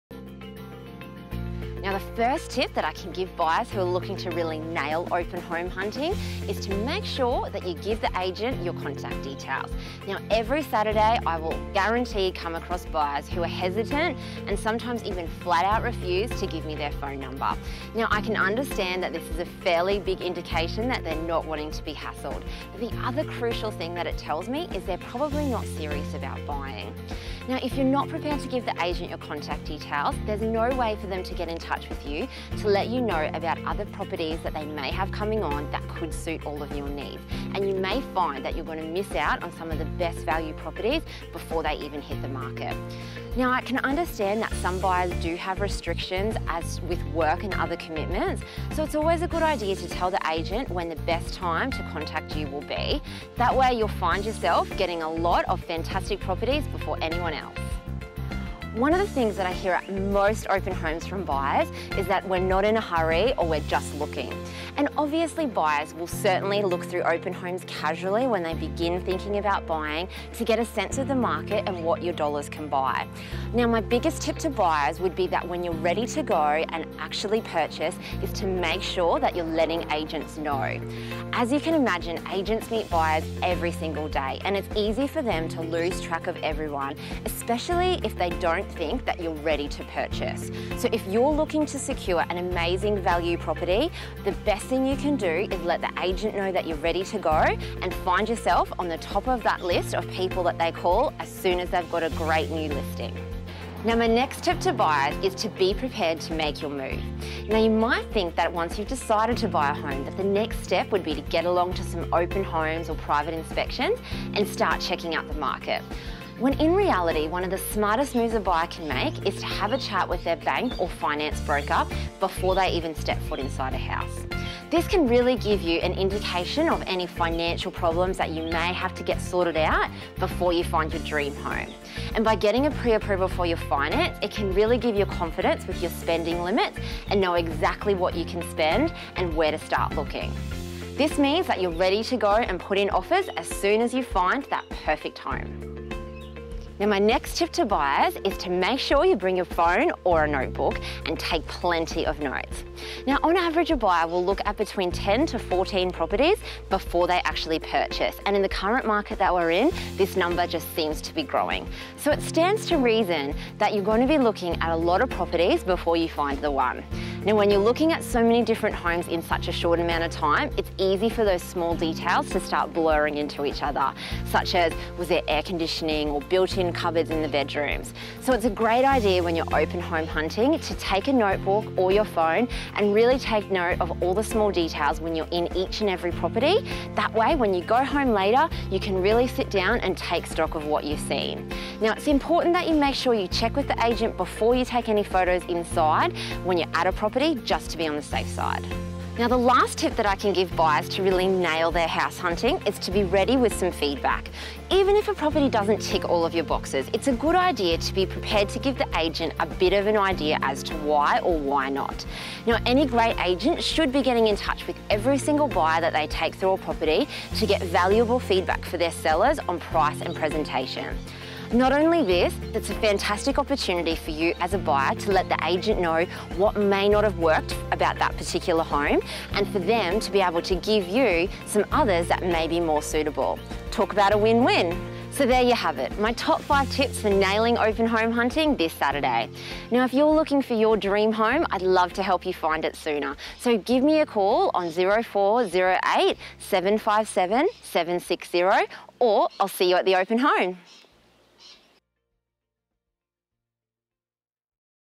Rockhampton Open Home Hunting with Talitha Dodson
Are you planning to head out house hunting this weekend?
If you’ve been in the market for a while or are only just starting your search, you’ll know that Saturday open homes are a real estate staple. Most Agents have back to back inspections arranged for the day with only a small travel window allocated between each 30 minute open. With this frantic pace to maintain it is easy for buyers to feel like they are not getting the level of attention that they deserve, but there are a few simple things that buyers can do to make the most of their Saturday mornings!